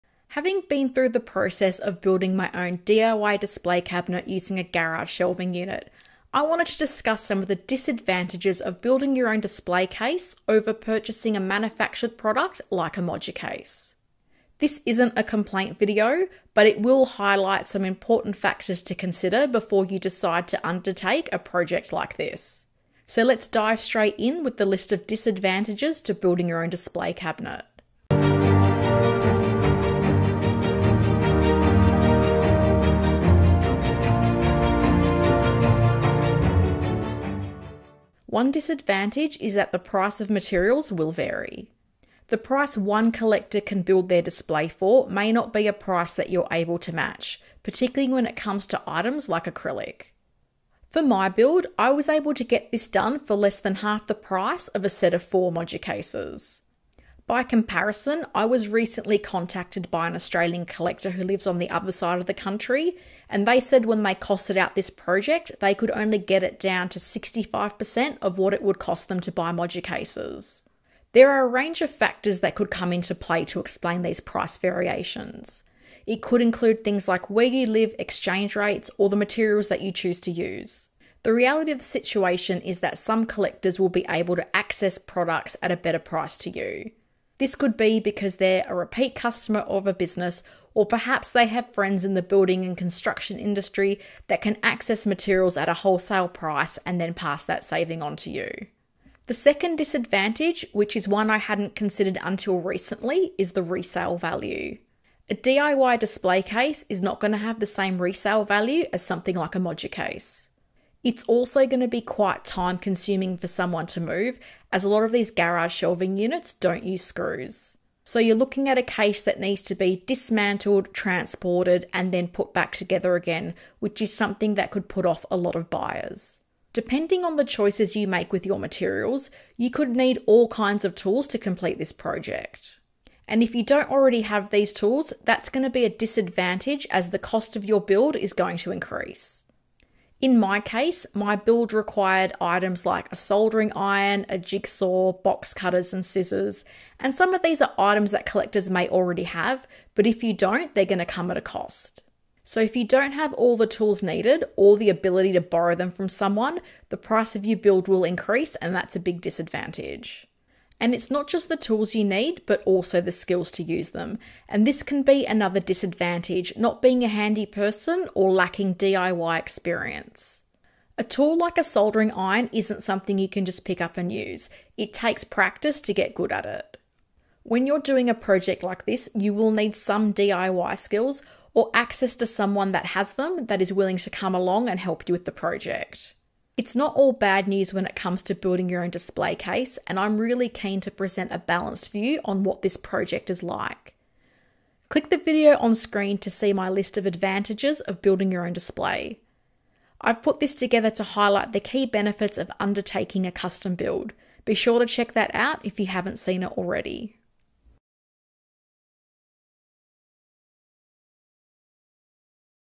Pitfalls of DIY Display Cases for Hot Toys Collectors
Discover the disadvantages of converting a garage shelving unit into a display case for your Hot Toys collection.

In this video, I address the key disadvantages of building a DIY display case compared with purchasing a manufactured product, such as Moducases/Moduspaces.

It’s not all bad news, though, so be sure to watch my video on the key advantages of building your own display case

If you’d like to find out more about my build and the materials I used, check out my original video